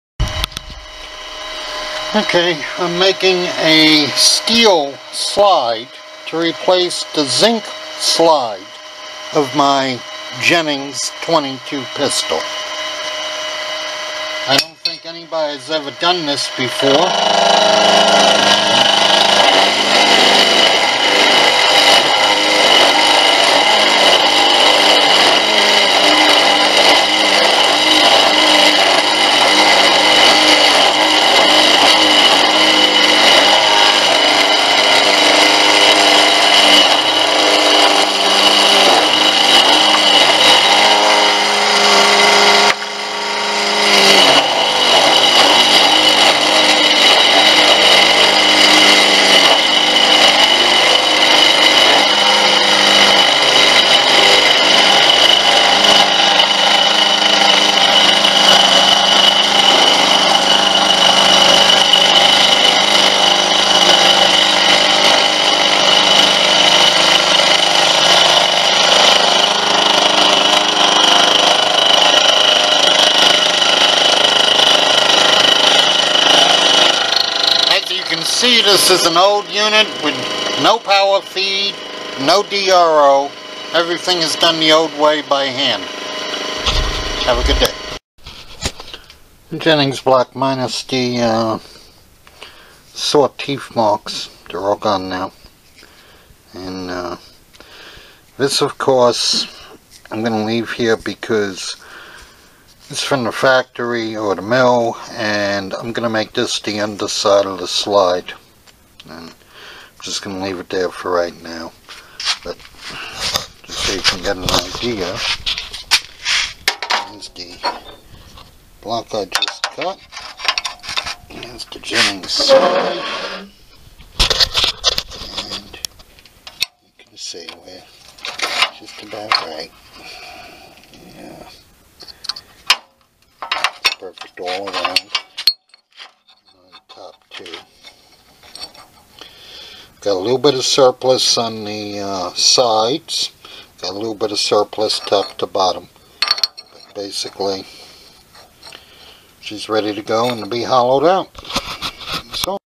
fabricating a steel Jennings J-22 slide
I get bored and realize many of the malfunctions of the Jennings J-22 class of weapon can be traced to the kong ago decision to hold down costs and weight by making both the slide and the frame out of a Zinc alloy.
That led to a what if moment, followed by a why not? moment.  A lot of Google showed no trace of any similar projects and only one other on YouTube that even come close.  Heck I couldn't even find drawings for a Jennings and had to start making my own.  It looks like a straight forward project, but there are complications.  The Jennings is a casting  Sone ingenuity will be needed to machine a drop in slide.  The nose of the slide will be the first challenge.
There is a .45 caliber recoil spring inside the .80 caliber slide.  Is .17 caliber on the left and right enough steel to install a threaded end cap?  What if I install a longer barrel and thread the slide retaining cap onto that?  Slowly I realize I am not wedded to the original shape.  What is the purpose of the slide?  To carry the fire pin?  Can I go open top like a Beretta Minx?
Questions, all things in time.  For now, I just do it.  Me and my Sieg Mill.  If I stumble, I stumble.  Already thoughts of how to make a steel frame swirl in my head.  The issue is converting a cast or forged design into a milled design.
ADDENDUM   After several 'tool room' prototypes were made and experimented with, a decision was made to suspend the project in 2018.